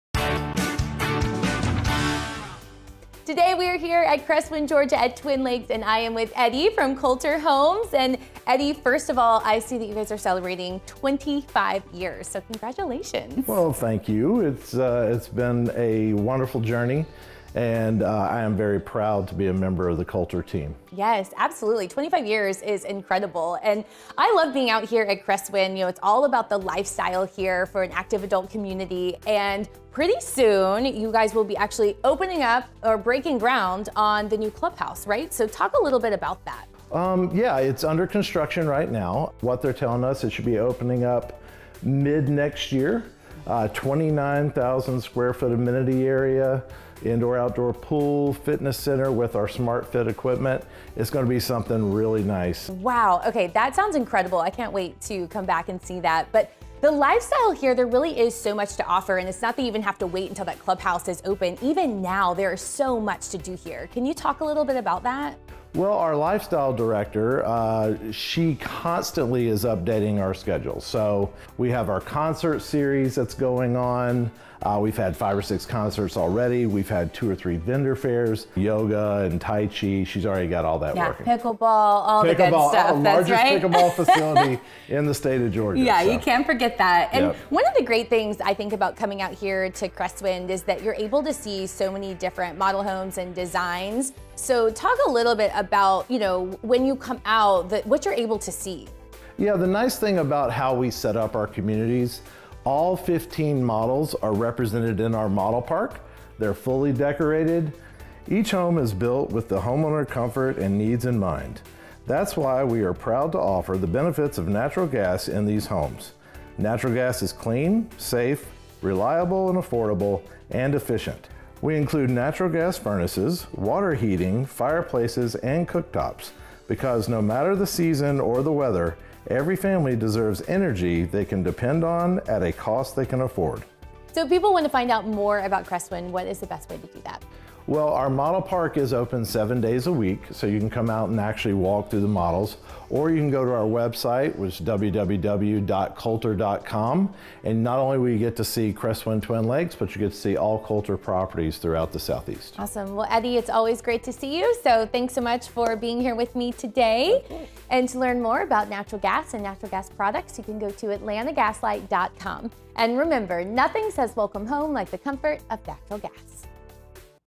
Cresswind Georgia At Twin Lakes - Atlanta Gas Light / Kolter Homes 2221
Cresswind Georgia At Twin Lakes - Kolter Homes
Hoschton/Braselton

Cresswind's active adult lifestyle is next generation fun & fitness designed for you. Cresswind Georgia residents enjoy access to a resident-exclusive clubhouse that features a SmartFit Training Center with cardio, weights and EGYM Smart Circuit Training, indoor pool with lap lanes, arts and crafts room, social ballroom and more. Outdoor amenities include Pickleball and Tennis courts, walking trails, a resort-style pool and event lawn with band shell. The full-time Lifestyle Director keeps the social and event calendar full of activities designed for the 55 Plus lifestyle.